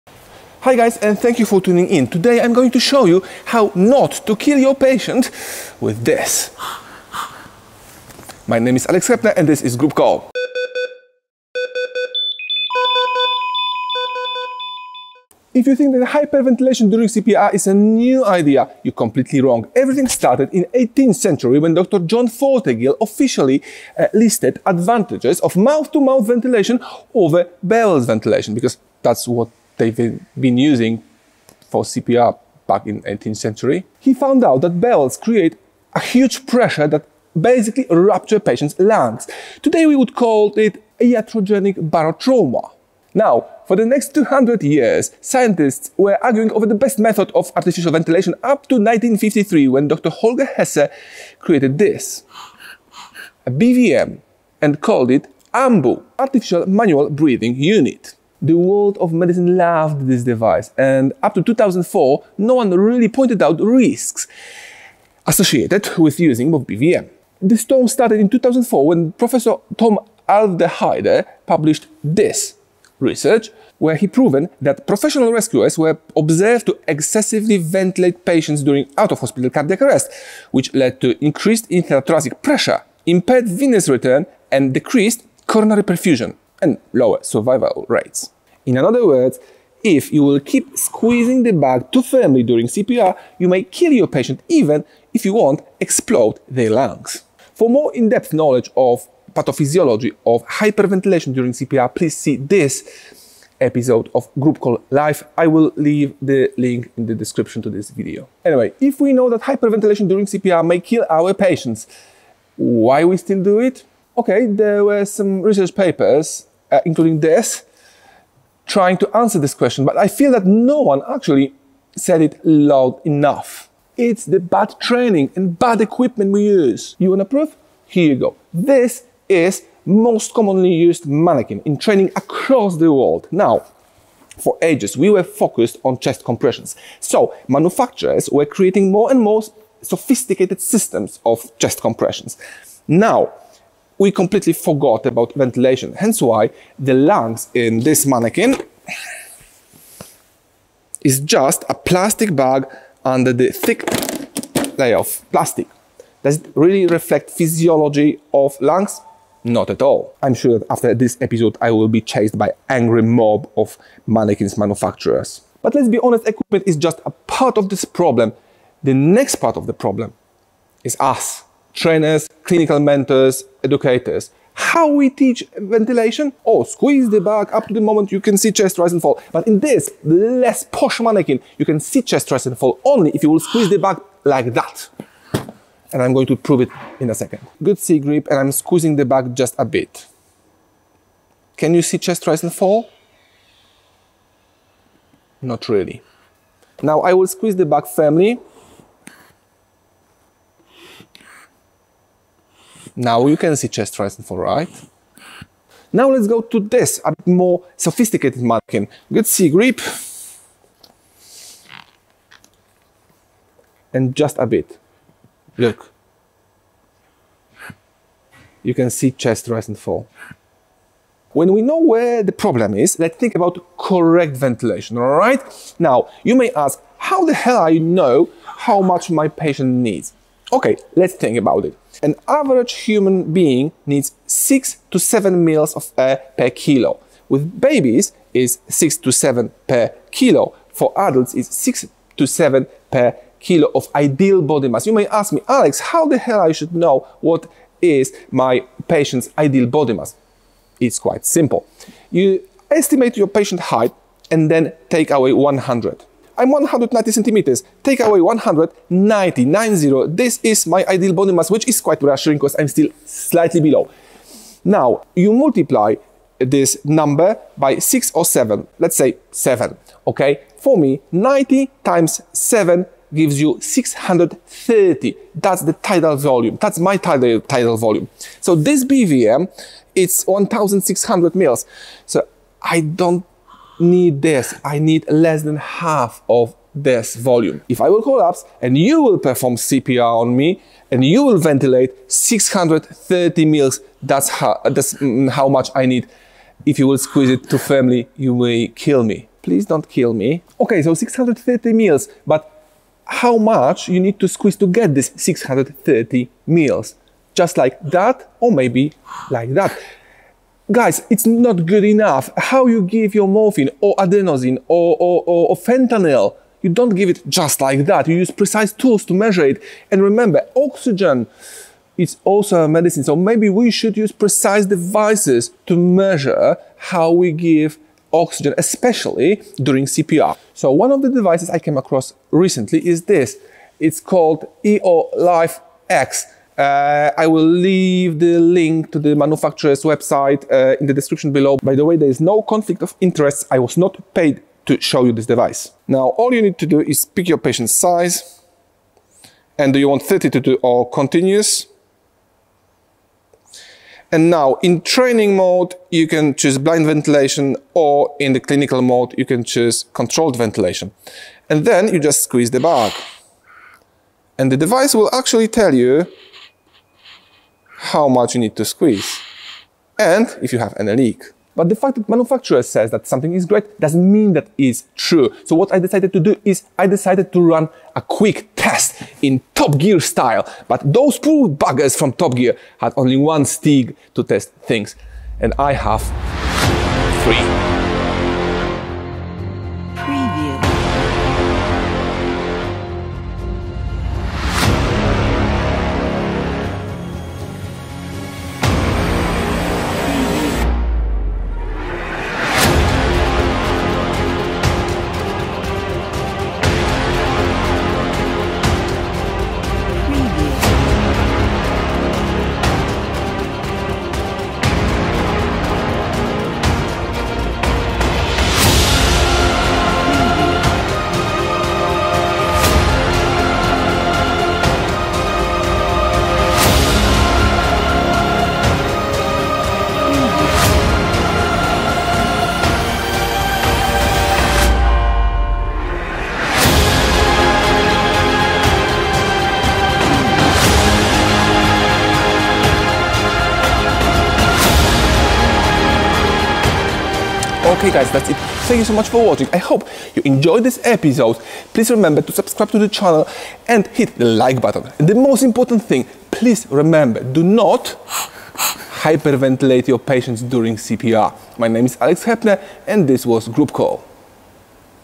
Hyperventilation during CPR
From the 18th century we know that hyperventilation may jeopardise the cardiopulmonary resuscitation, so why we are still killing our patients by squeezing the bag too firmly? And how we can prevent this?

A. Khoury: Evaluation of BVM Ventilation in Manikin Studies

A. De Luca: Reliability of manikin-based studies: an evaluation of manikin characteristics and their impact on measurements of ventilatory variables

A. Khoury: Ventilation feedback device for manual ventilation in simulated respiratory arrest: a crossover manikin study